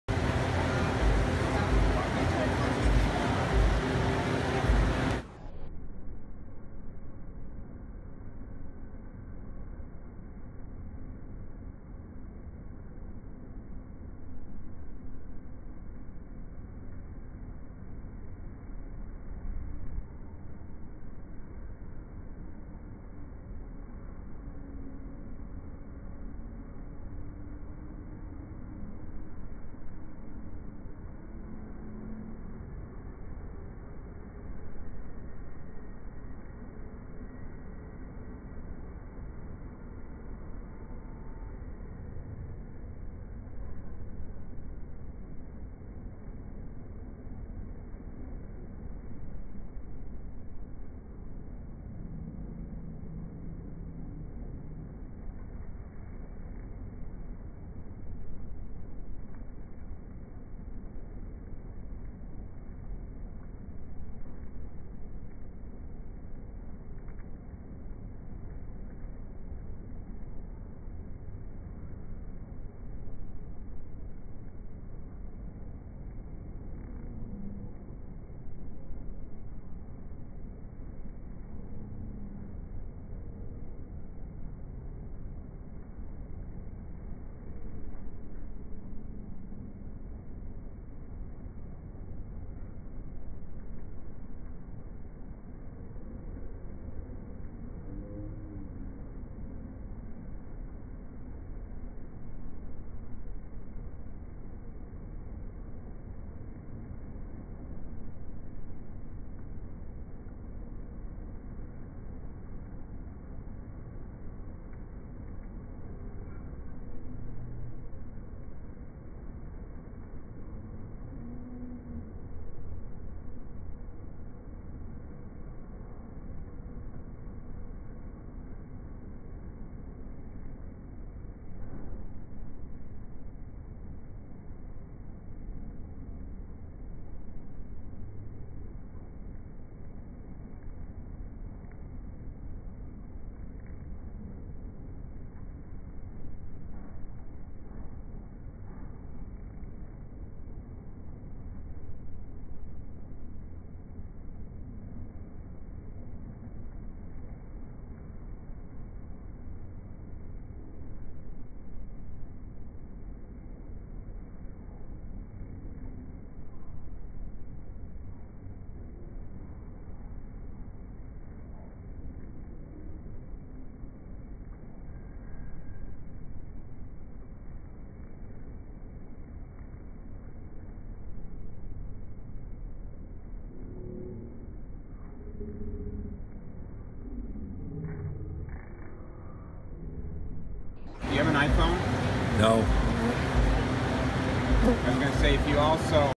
Slo-mo Bees 5421
Slo-mo video of honey bees enjoying Zemer's Home-Made root beer at the Chocolate Affair in Glendale, AZ 2018.